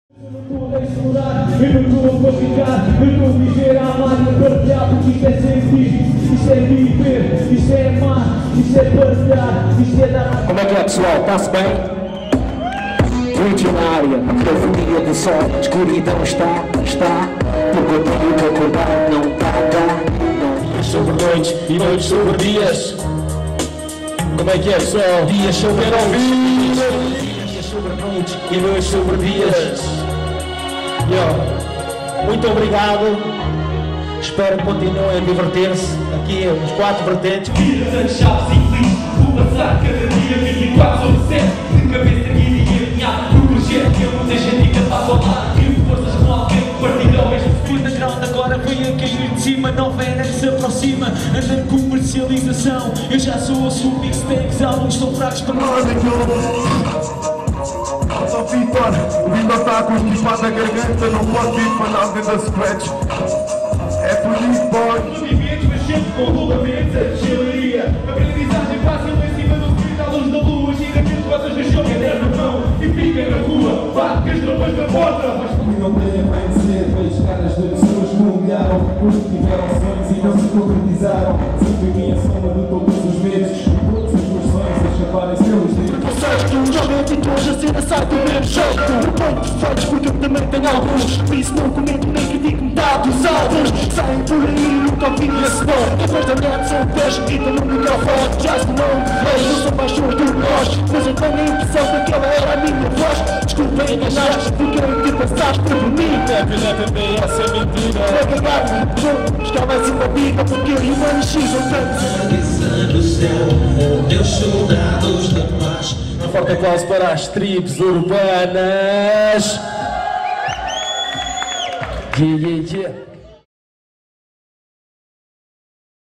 4 Vertentes: o festival que celebra o hip hop da zona centro
Pelo terceiro ano consecutivo, Leiria recebeu a festa do hip hop da zona centro do país. 4 Vertentes celebra o breakdance, graffiti, dj e mc que se faz por cá. Este ano a festa foi no Mercado de Santana, por onde passaram centenas de apreciadores e curiosos.